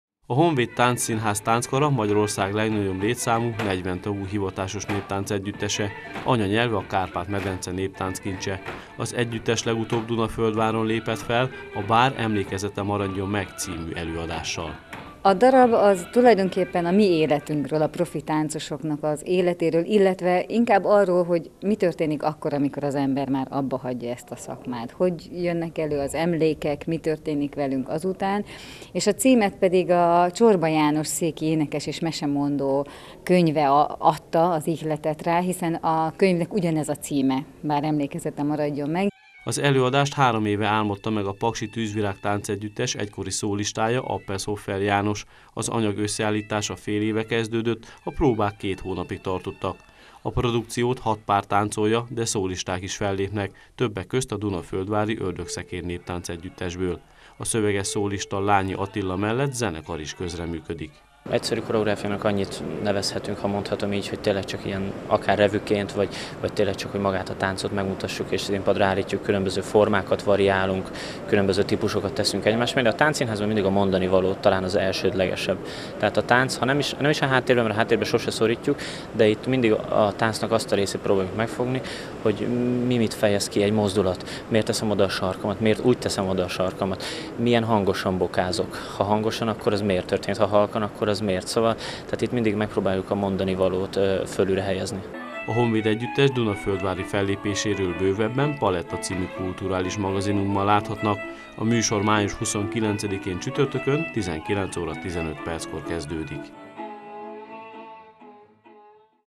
Dunaföldváron lépett fel a Honvéd Együttes
A Honvéd Együttes jelenlegi és volt szólistái léptek színpadra a Dunaföldvári Művelődési Központban a "Bár emlékezete maradjon meg" című előadással. A produkció koreográfusa a paksi Tűzvirág Táncegyüttes egykori szólistája, Appelshoffer János.